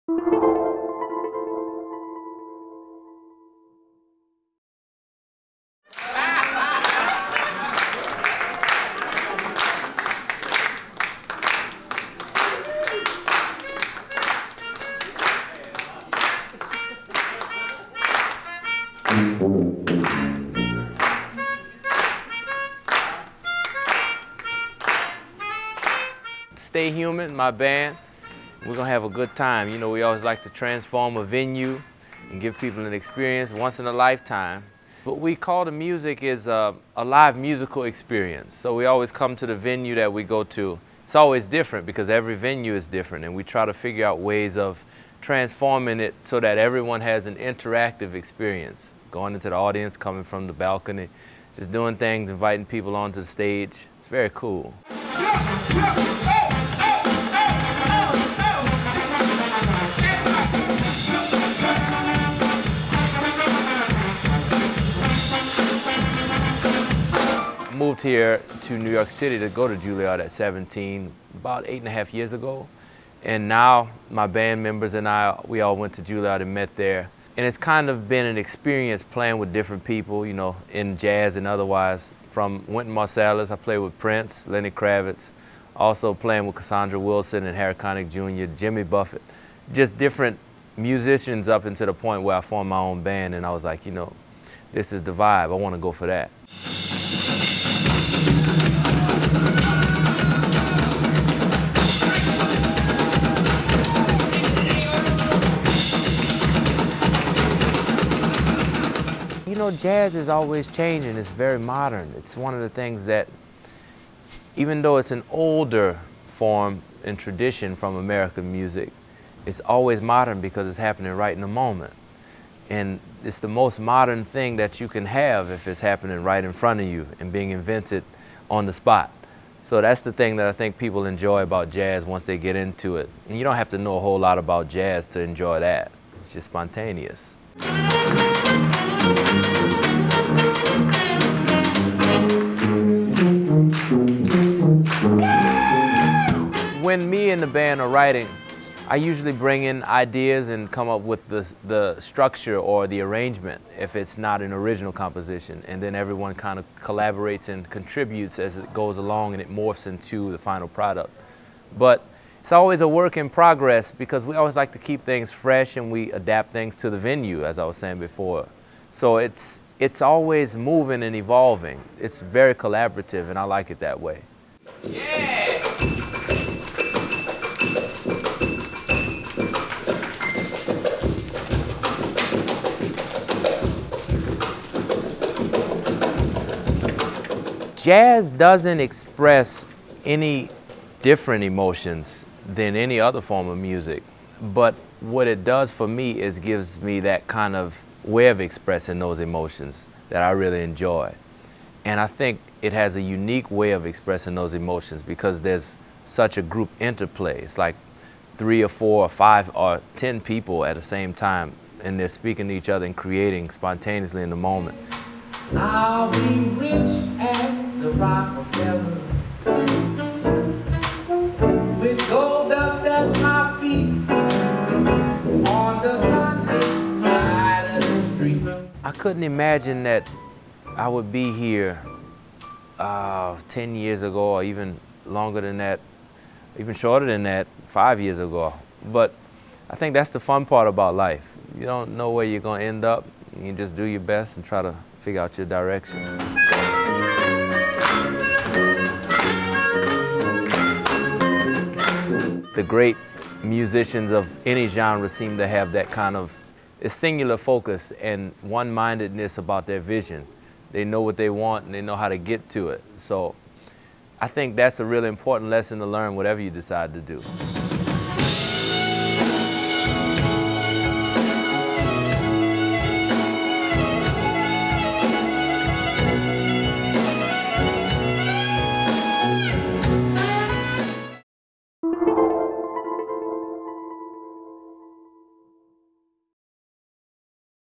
Jon Batiste's Jazz Performance Transforms the Landmark on Main Street in Port Washington, NY
The Landmark on Main Street presents a jazz concert with Jon Batiste.

Featured Interviews: Jon Batiste, Jazz Artist